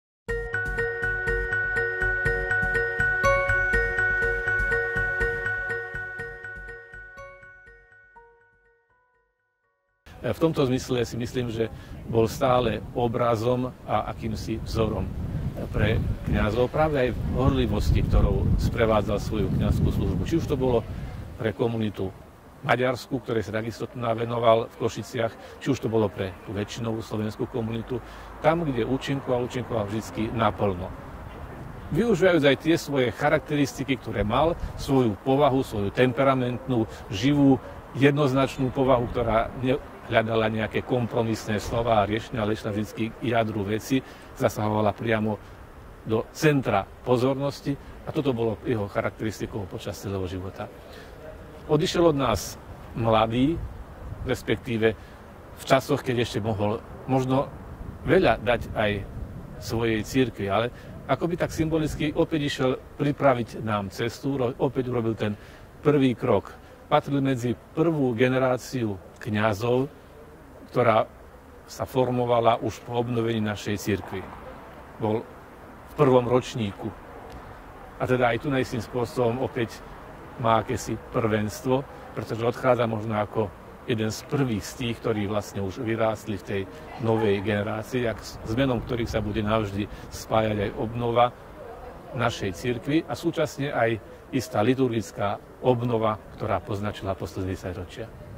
VLADYKA CYRIL VASIĽ SJ SI SPOMÍNA NA ZOSNULÉHO PROF. VOJTECHA BOHÁČA
S jeho menom sa bude navždy spájať obnova gréckokatolíckej cirkvi. S týmito slovami si na nedávno zosnulého kňaza prof. Vojtecha Boháča spomína vladyka Cyril Vasiľ SJ.